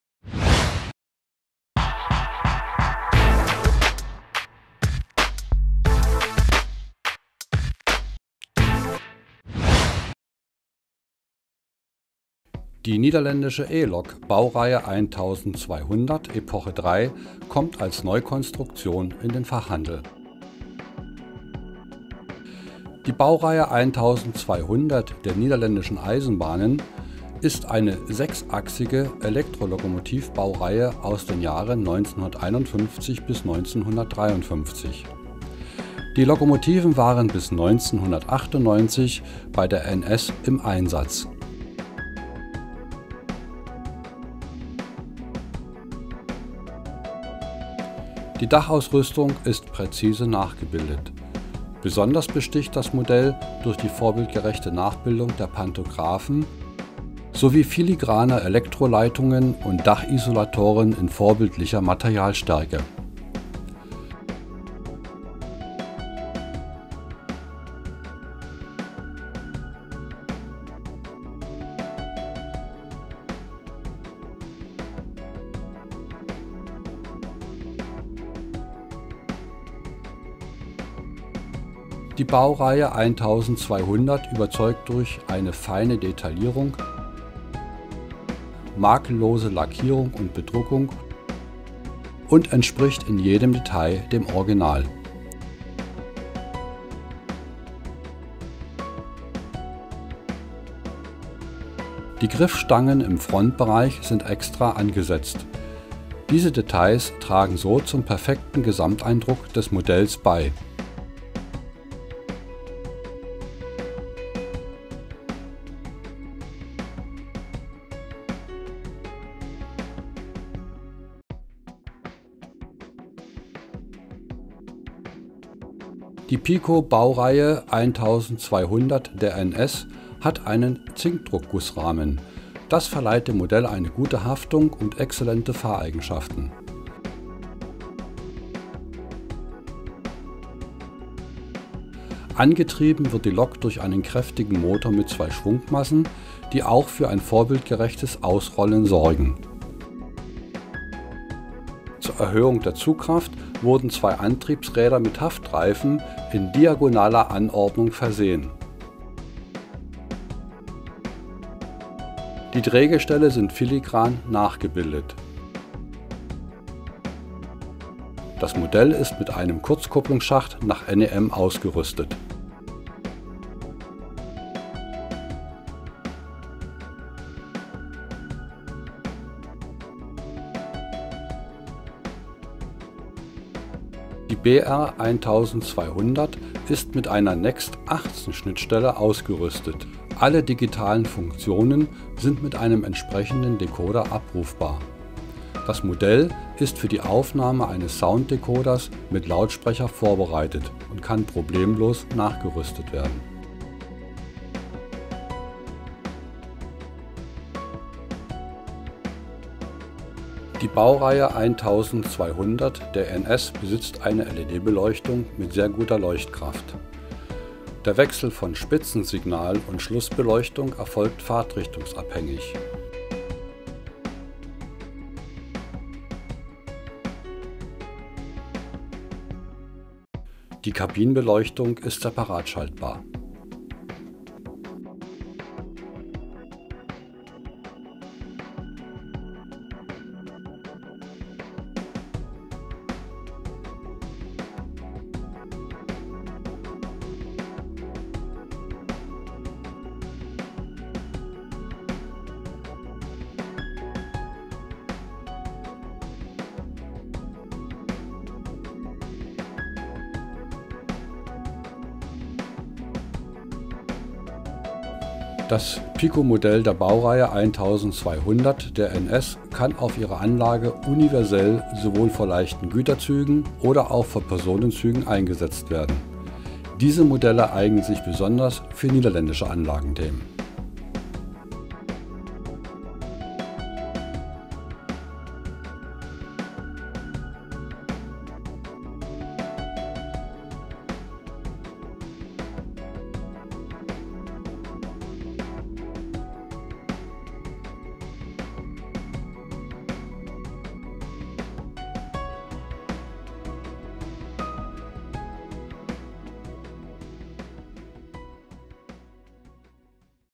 PIKO Promovideo -offiziell- | N E-Lok 1200 (40460)
Das offizielle PIKO Promovideo zur formneuen N E-Lok 1200 NS 404060

Fertiges Serienmodell - kein Handmuster.

Zur Verfügung gestellt durch PIKO Spielwaren GmbH